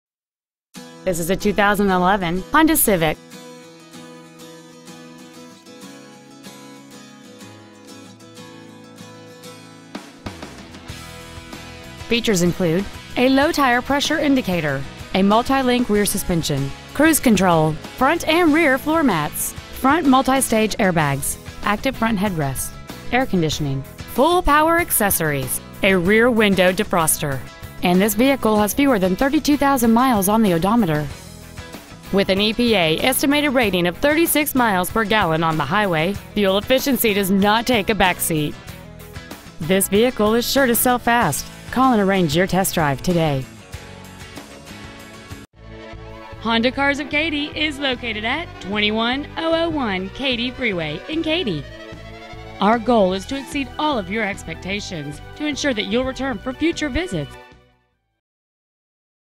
2011 Honda Civic Cpe Katy TX
We are proud to present this 2011 Honda Civic Cpe .

 Year : 2011
 Make : Honda
 Model : Civic Cpe
 Engine : 1.8L SOHC MPFI 16-Valve i-VTEC I4 Engine

 Miles : 31,467

 Preowned 2011 Honda Civic Cpe Katy TX